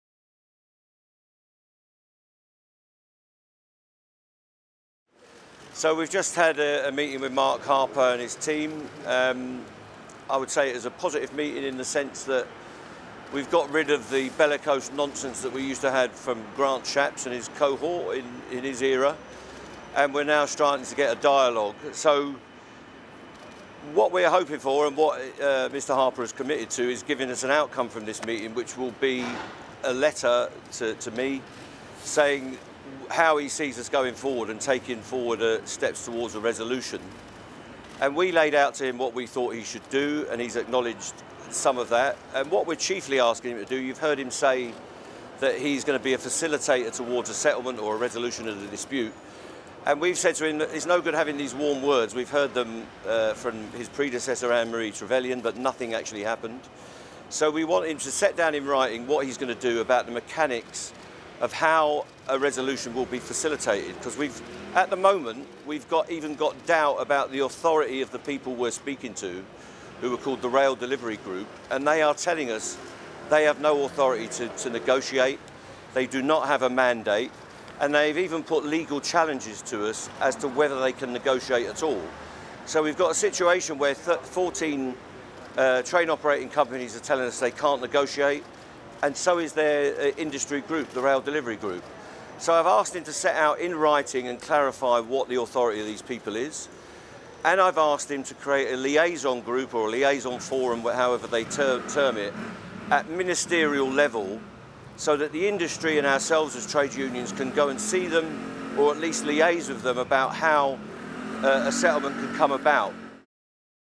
Mick Lynch: Talks with Transport Secretary were positive
Train strikes around Christmas will not be called off until a solid settlement has been reached with rail companies, union boss Mick Lynch insisted on Thursday.